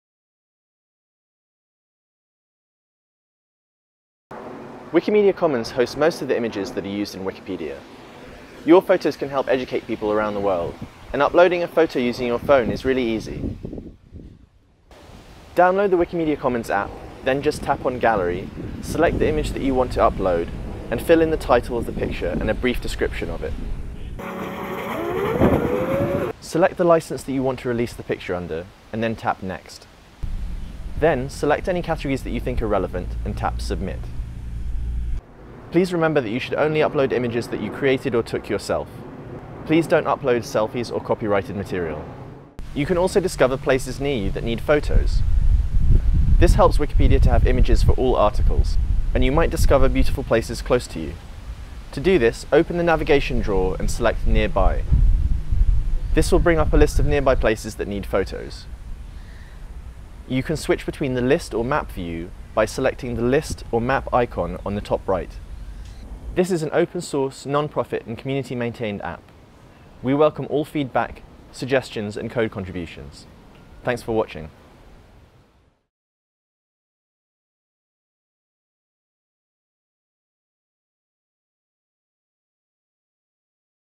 Wikimedia Commons mobile App
A demonstration of some of the features of the Wikimedia Commons mobile app.